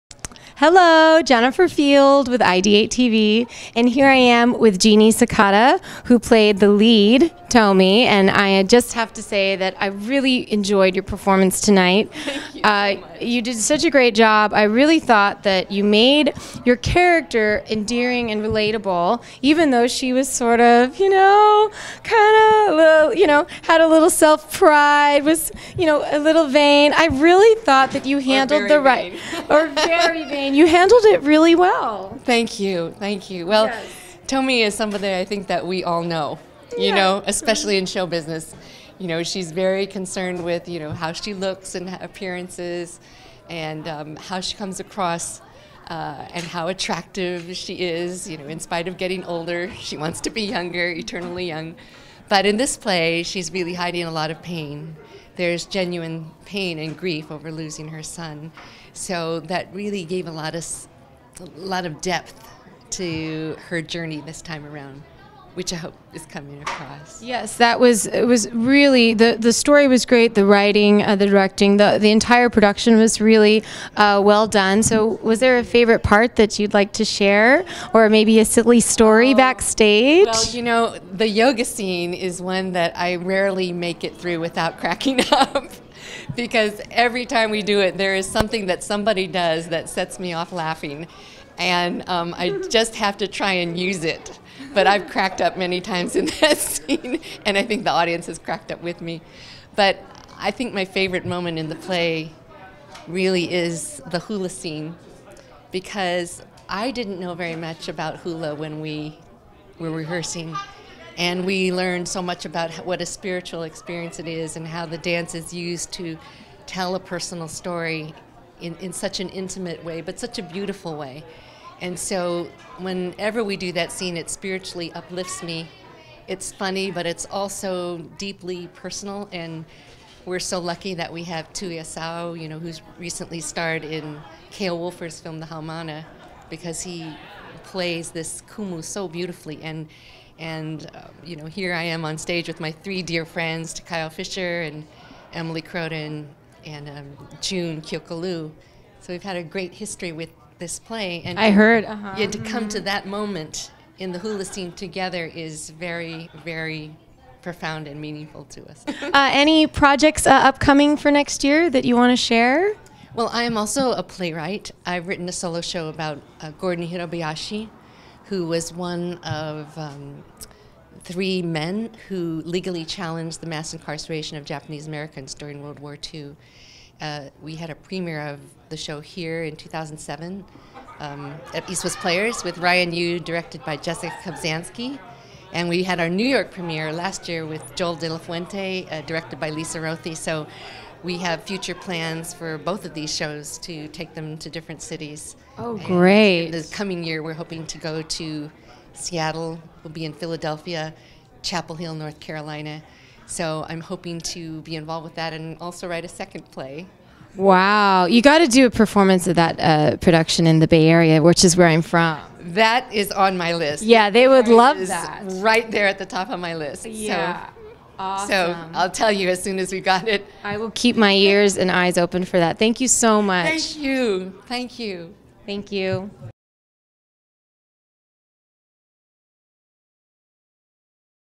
EWP Nisei Widows Club with Jeanne Sakata - IdeateTV
Opening night for "Nisei Widows Club - How Tomi Got Her Groove Back" at East West Players. Host Jennifer Field interviews actress Jeanne Sakata. For more about the play and East West Players please